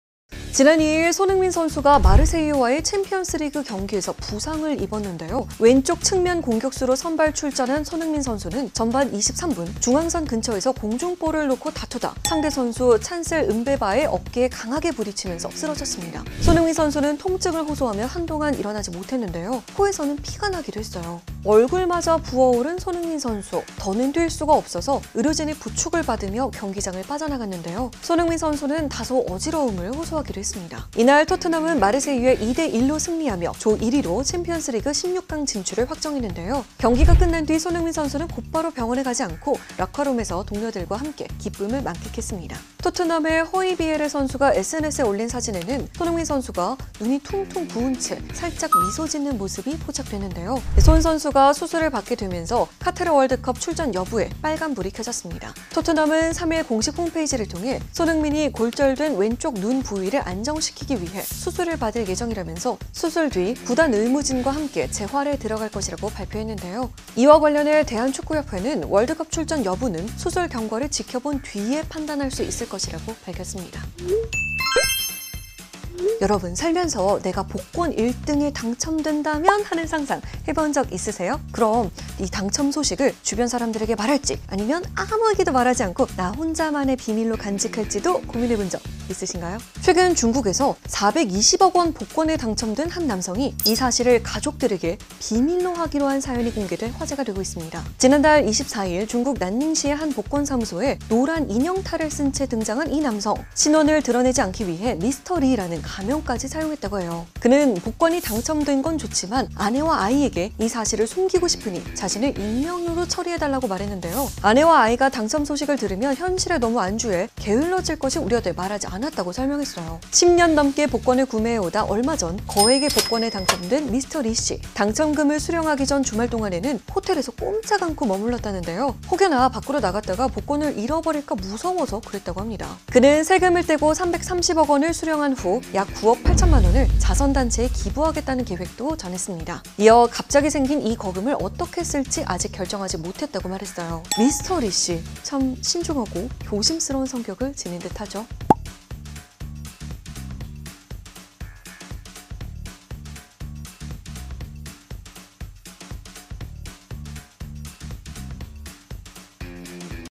"손흥민 안면 골절로 수술” 월드컵 출전 가능할까? 축구 협회 입장은? / 14F
토트넘 손흥민 월드컵 뉴스 "손흥민 안면 골절로 수술” 월드컵 출전 가능할까? 축구 협회 입장은? / 14F 토트넘,손흥민,월드컵,뉴스